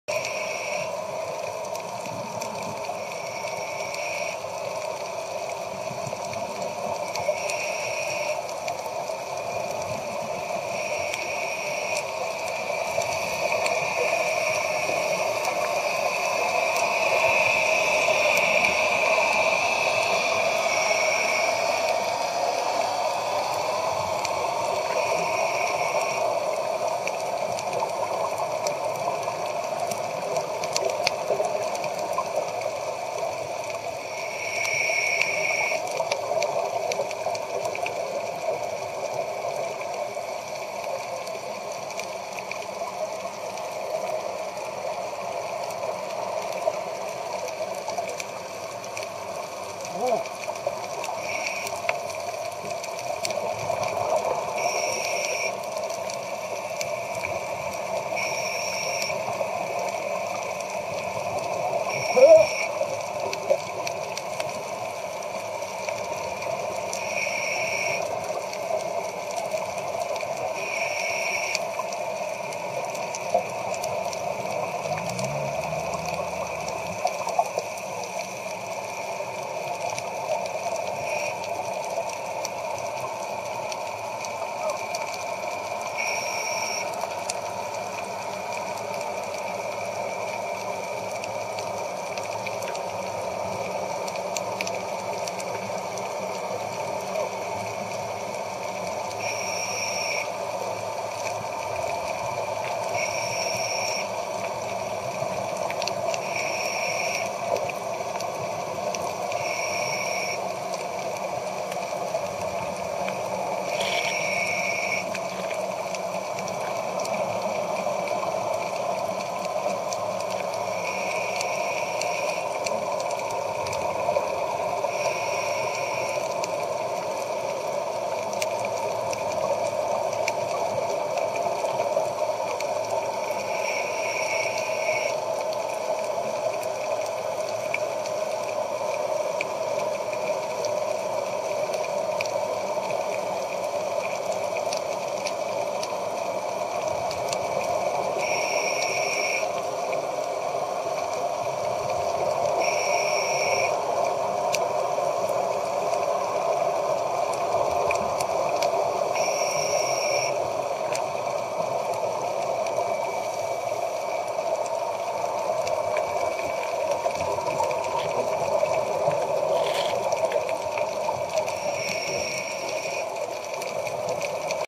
Frog Fish St.Andrew underwater
Sunday 31 July 2011 I meet SCUBA friends while swim around jetties in Sant Andrew park. They show me hidden "Frog-Fish"
Camera; Sanyo HD 1010
Music; noise from Ocean